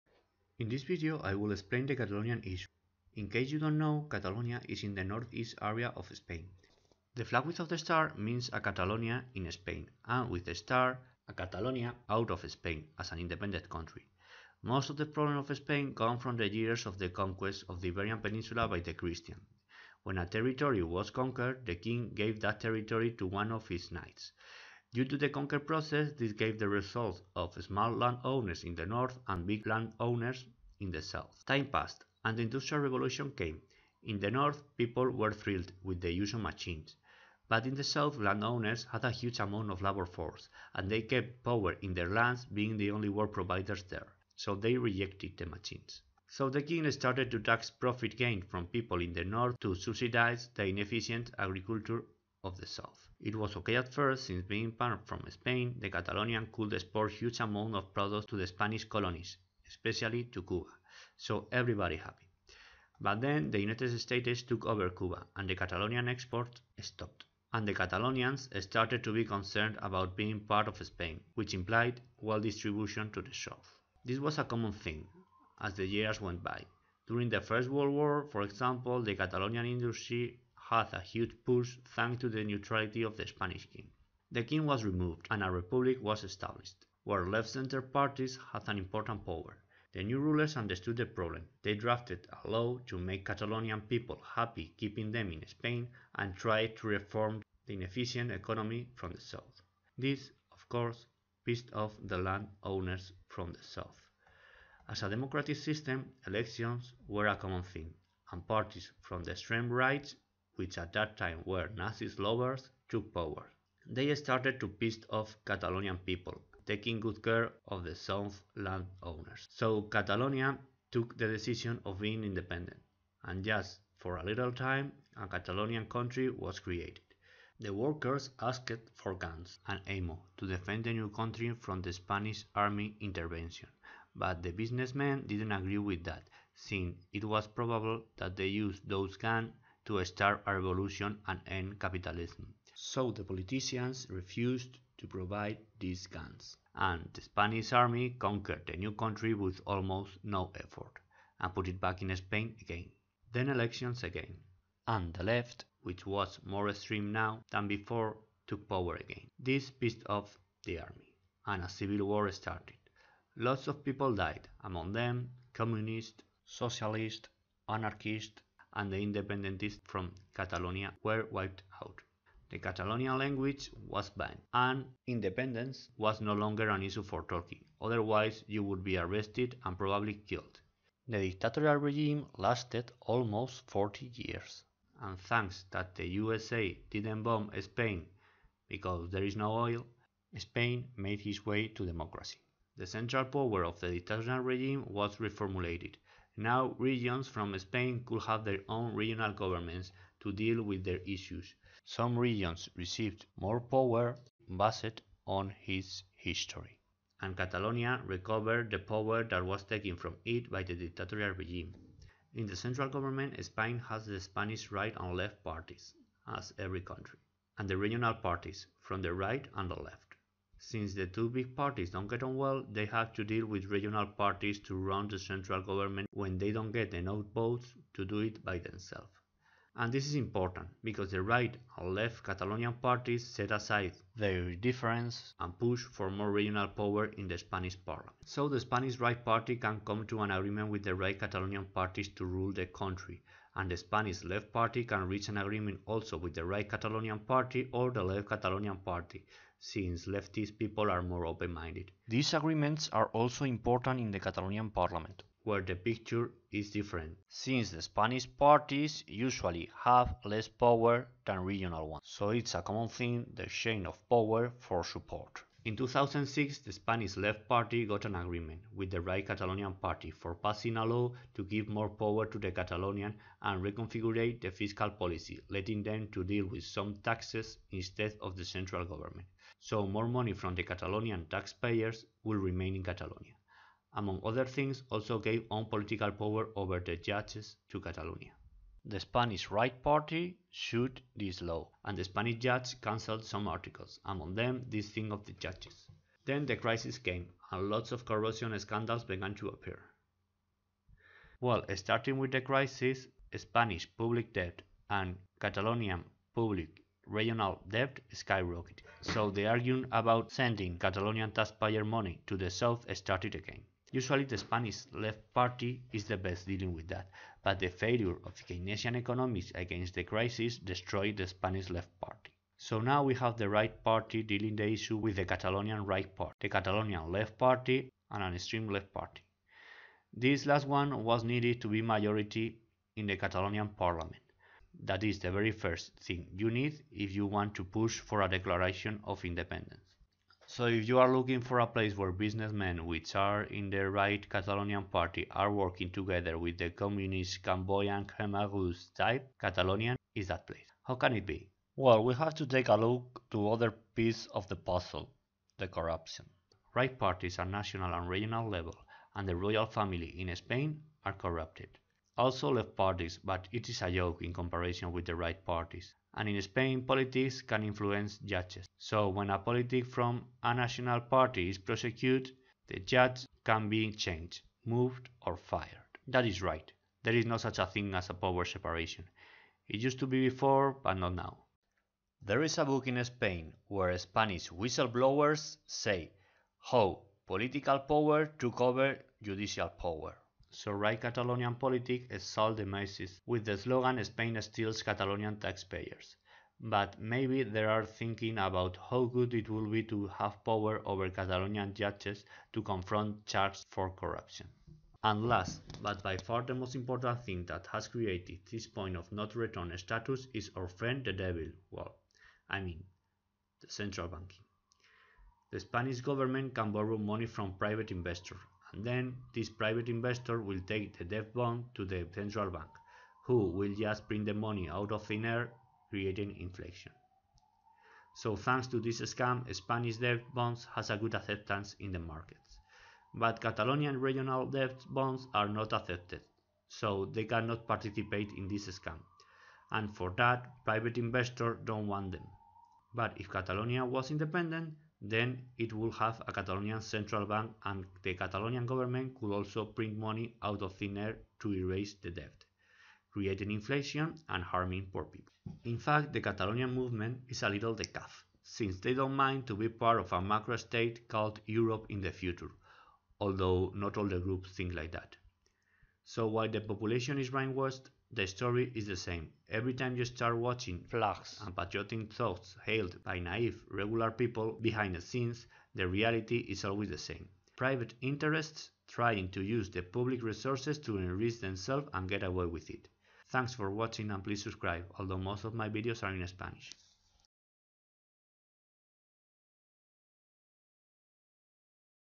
The Catalonian issue. Explained for non Spanish people.
In this video we explain the catalonian issue from scratch. You don't even need to know where Spain is to understand the video.